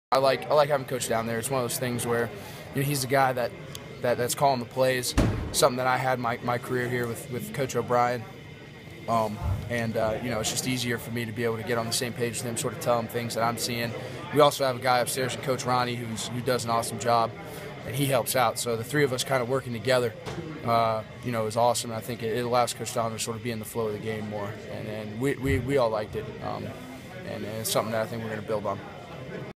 Penn State football: Christian Hackenberg talks about having offensive coordinator on the sidelines
Penn State quarterback Christian Hackenberg asked offensive coordinator John Donovan to move out of the coaches' box and onto the sidelines so that he would have easier access to him during games.